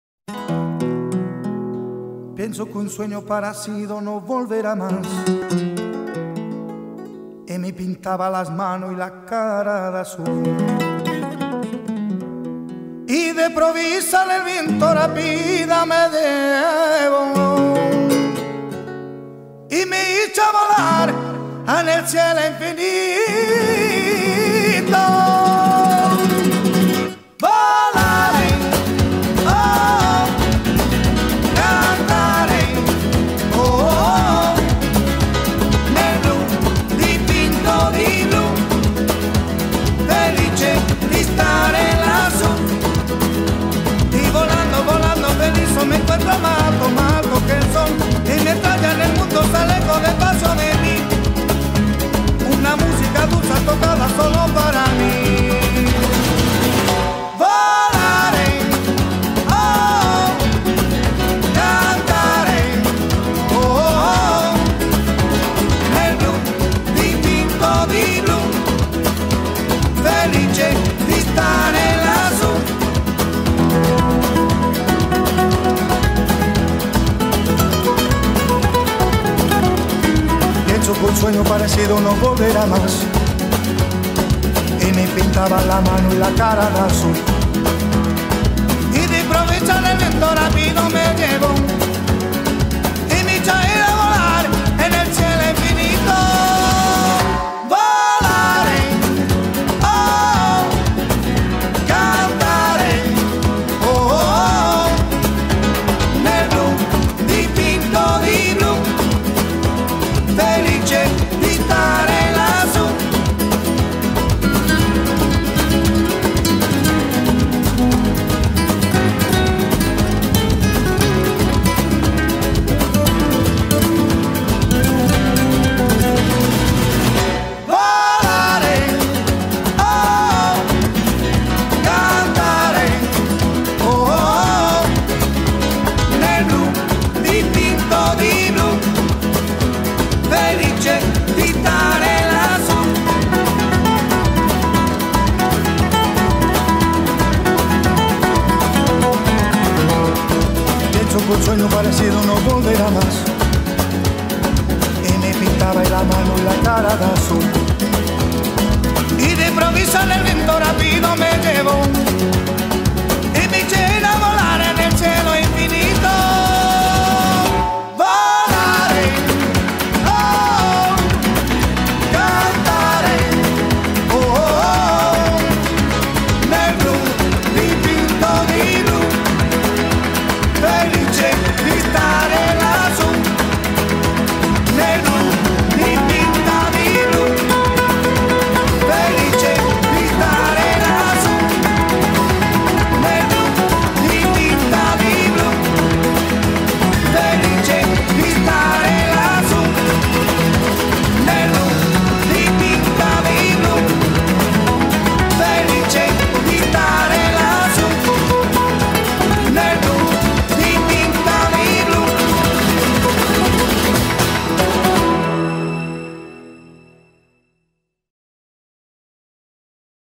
Volare
giro aereo su una zona di Roma